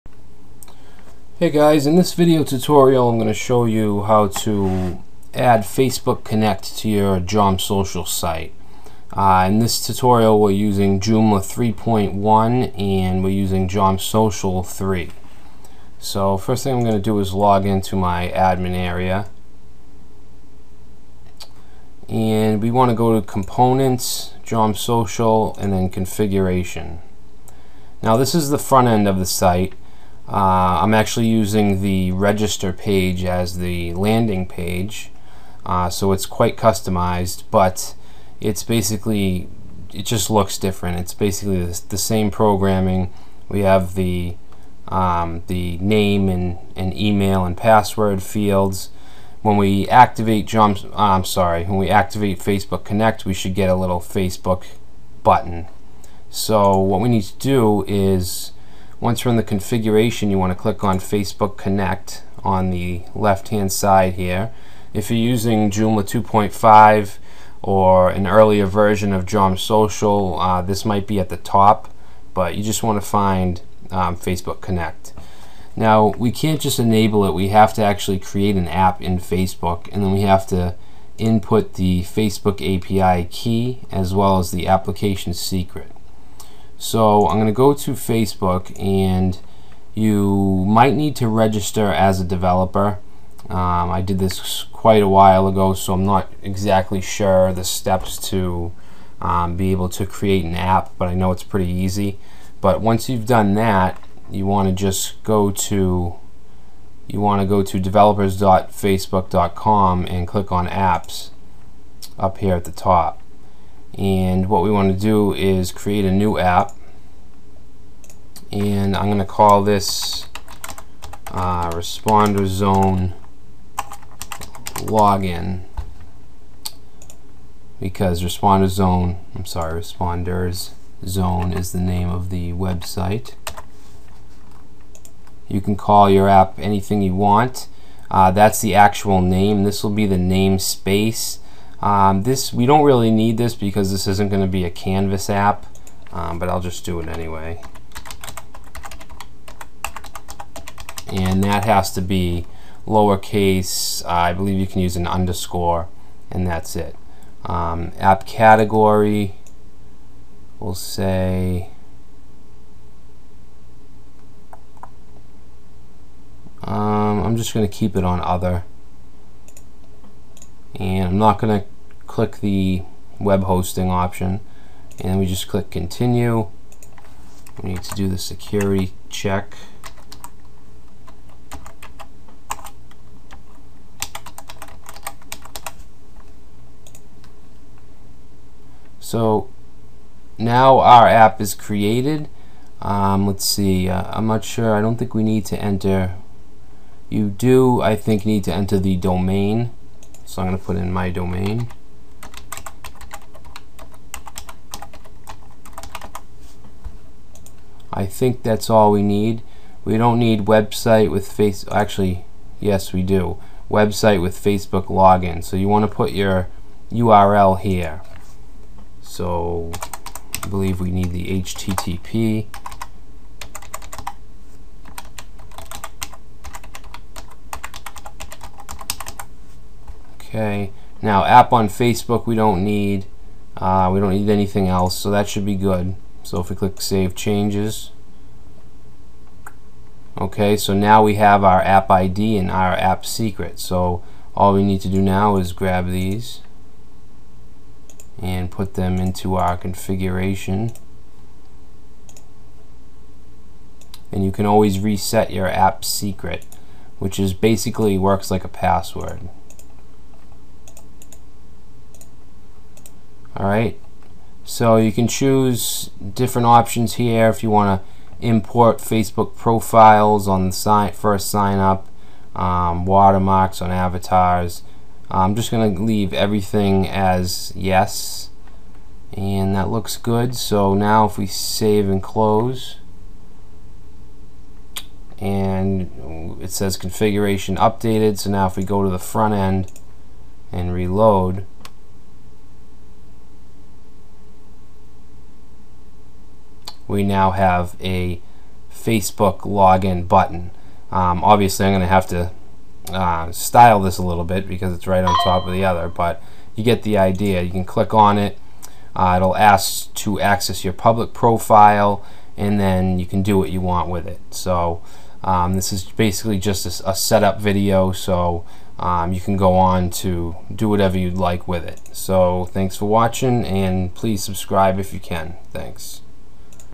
Add Facebook Login To Jomsocial
Quick video explaining how to add Facebook login/connect to your Jomsocial website.